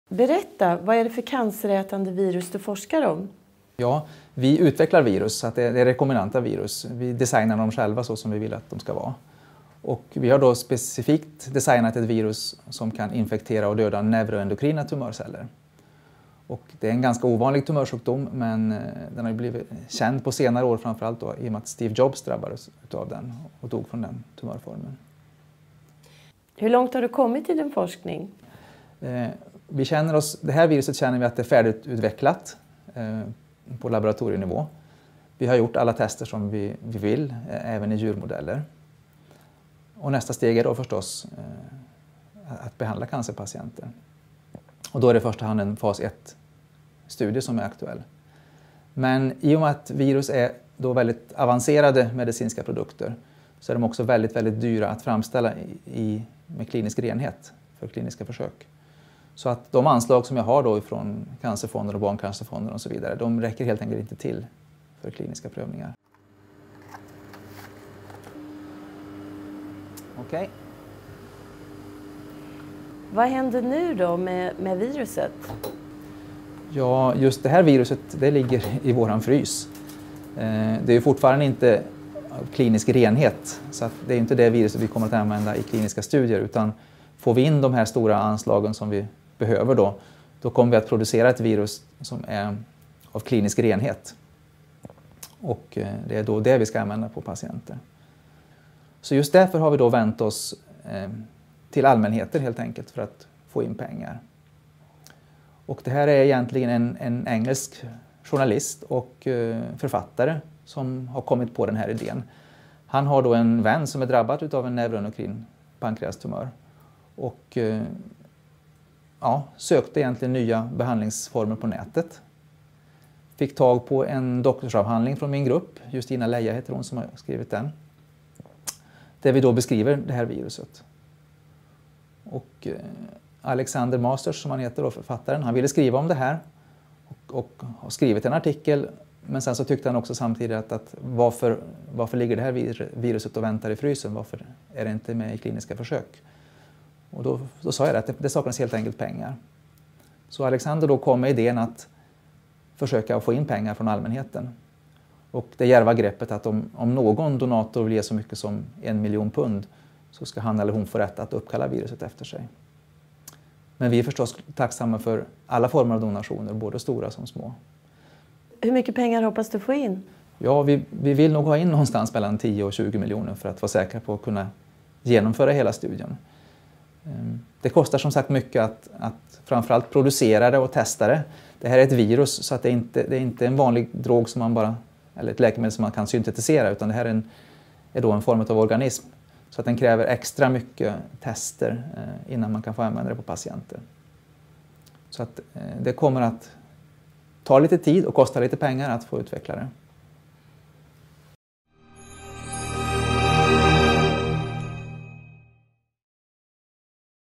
Professor Magnus Essand berättar om sin forskning om onkolytiska virus - Uppsala universitet
Uppsalaforskaren Magnus Essands forskning om behandling av cancer med så kallade onkolytiska virus har uppmmärksammats stort i olika medier världen över. En fond - Onkolytiska virusfonden (OncolyticVirusFund) har upprättats vid Uppsala universitet till förmån för fortsatt forskning och kliniska tester.

Magnus Essands forskargrupp har utvecklat en virusstam som visat sig fungera  mycket framgångsrikt mot neuroendokrina tumörer hos möss. Forskningsrönen är publicerade i en rad studier. För att komma vidare behövs noggranna kliniska tester, vilket det inte funnits finansiering för.

Se även Uppsala Akademiska sjukhus Centre of Excellence for Endocrine Tumours